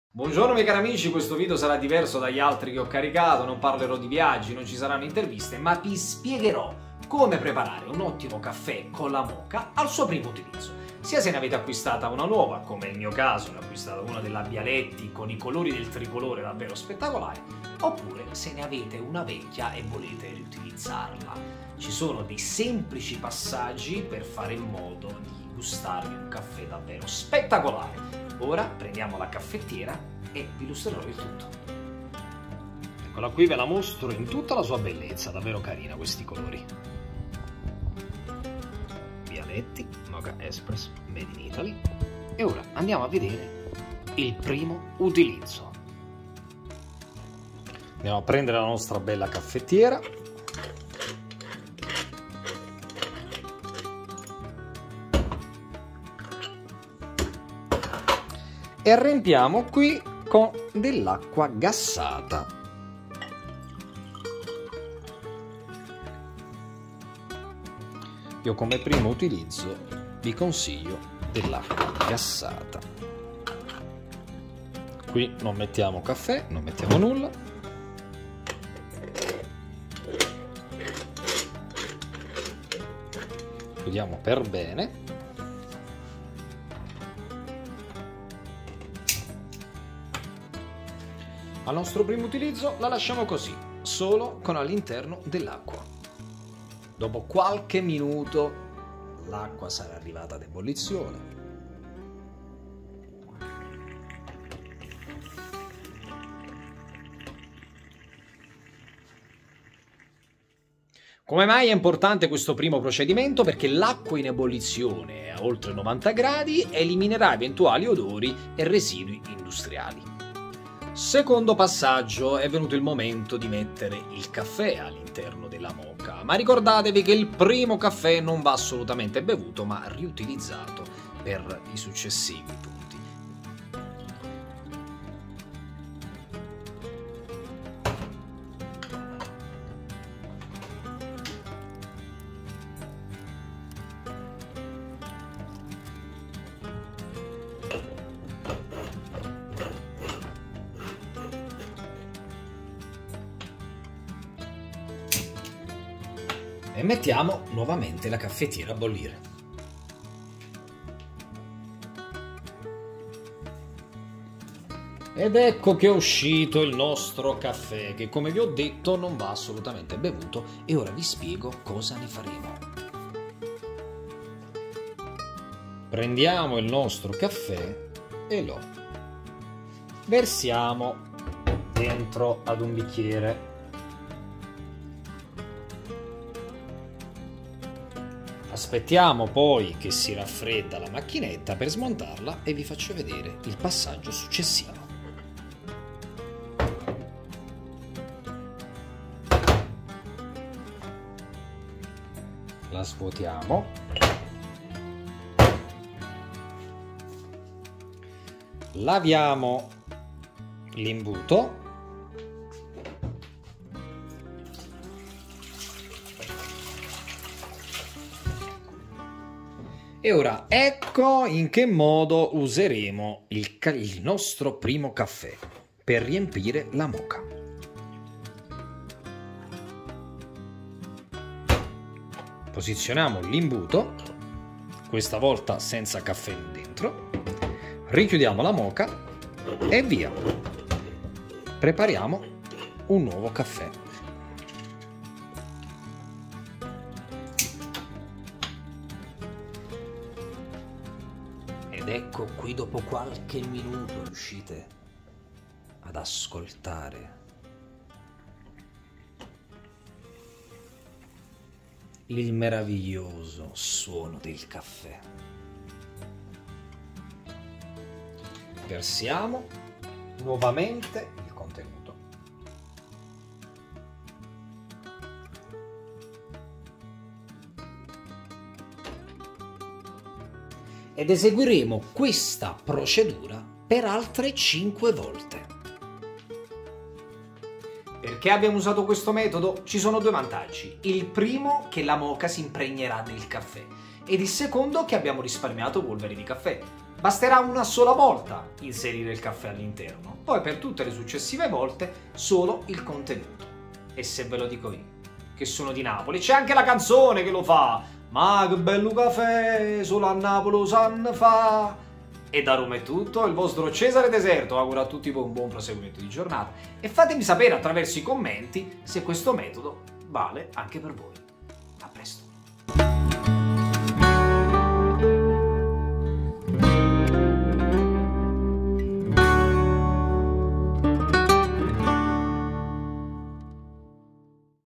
Moka nuova cosa fare al primo utilizzo (tecnica segreta)
Hai comprato una nuova moka e non sai bene quanti caffè ci vogliono prima di poterla utilizzare? Oppure hai a casa una macchinetta che non usi da un po’ di tempo e hai voglia non solo di riprenderla in mano ma anche di fare un’ottimo caffè. In questo video ti spiego una tecnica segreta (ho visto che non si trova online) per far partire alla grande la tua nuova o vecchia moka ed inoltre si tratta di un metodo che ti farà sprecare pochissimo caffè.